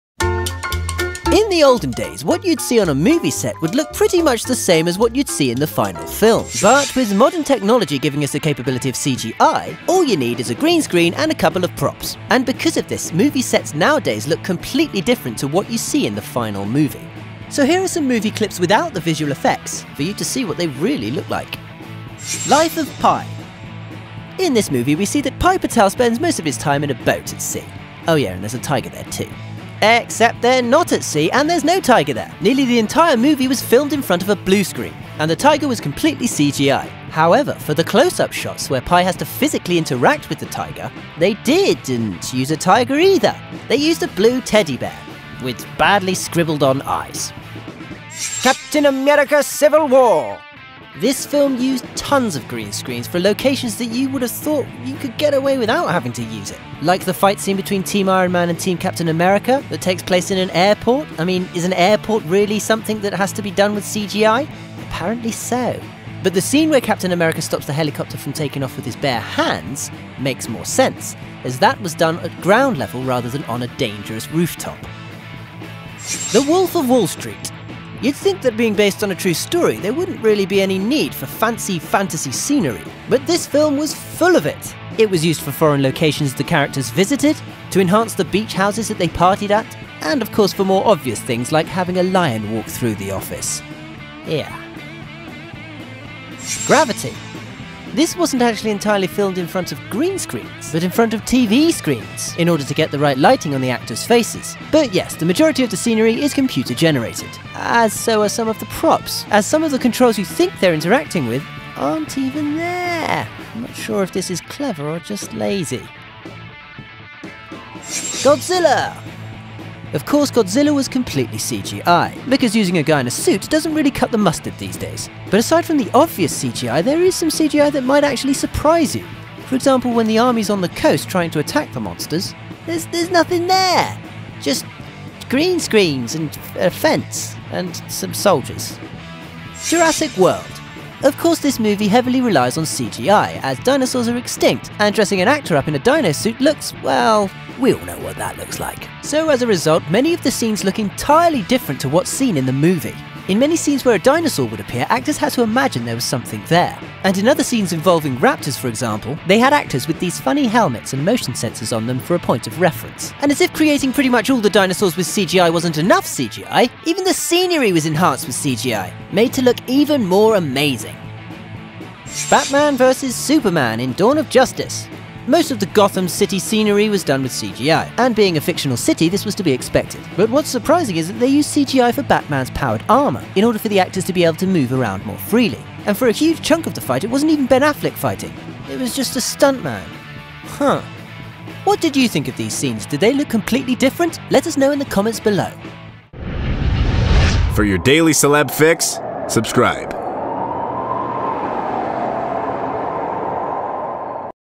All Hollywood VFX Removed! What Movies Really Look Like | How to make movie
It used to be that what you saw on a movie set would pretty much be what you saw in the final movie. If you wanted to have a mountain in the background, you'd have to go to one. However with modern technology, CGI has made it possible to have that mountain in the background without even leaving your home. Just whip up a green screen and you're good to go! This advancement in technology has meant for some big differences between what is filmed and what we see in the final movie.